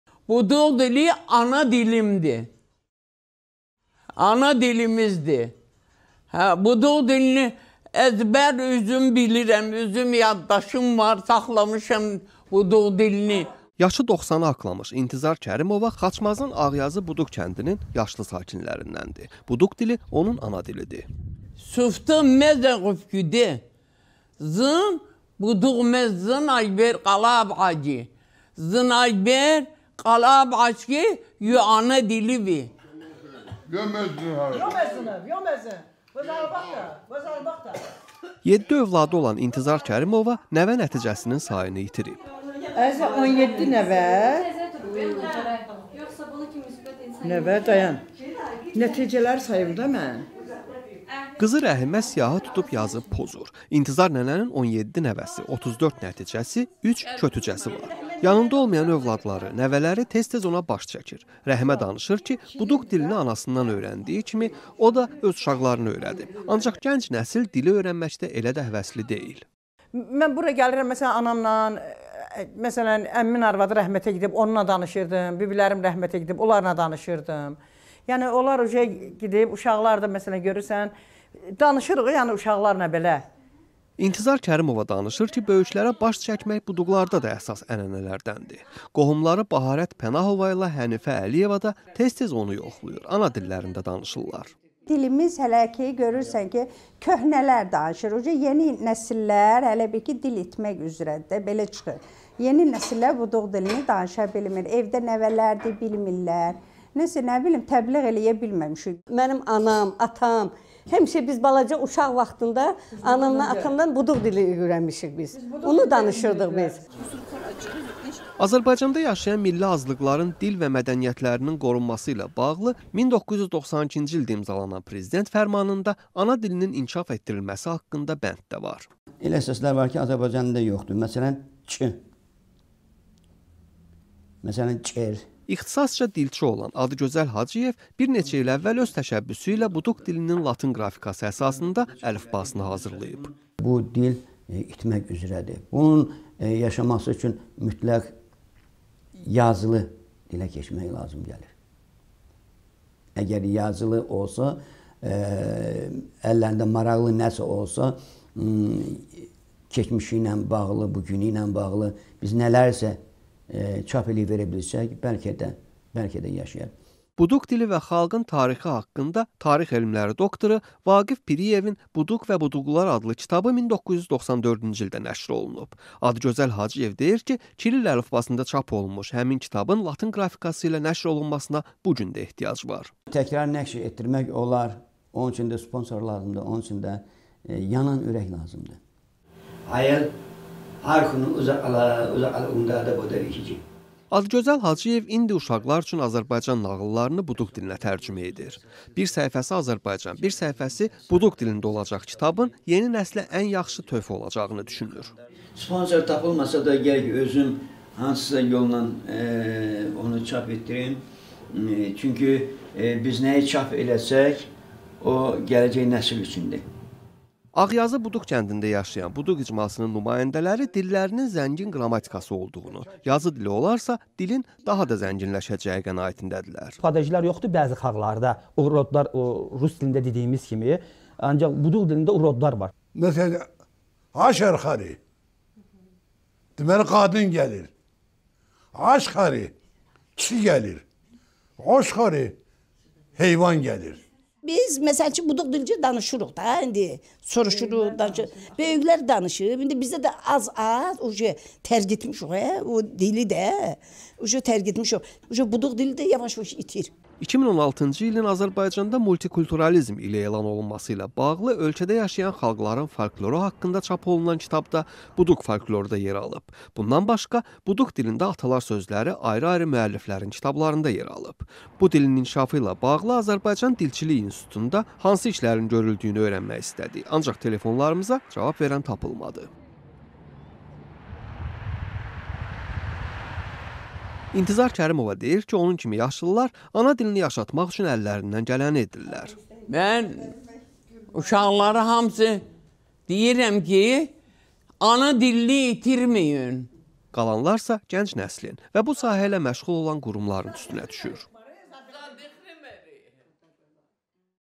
Buduq dili - İtməkdə olan dilin hekayəsi
Keçəlin nağılını başqa hansı dildə oxumaq olacaq? AzadlıqRadiosu itməkdə olan dilin hekayəsini təqdim edir. 21 fevral - Beynəlxalq Ana Dili Günü kimi qeyd edilir. AzadlıqRadiosu Xaçmazın Ağyazı-Buduq kəndinin sakinləri ilə onların itməkdə olan buduq dili ilə bağlı söhbət edib.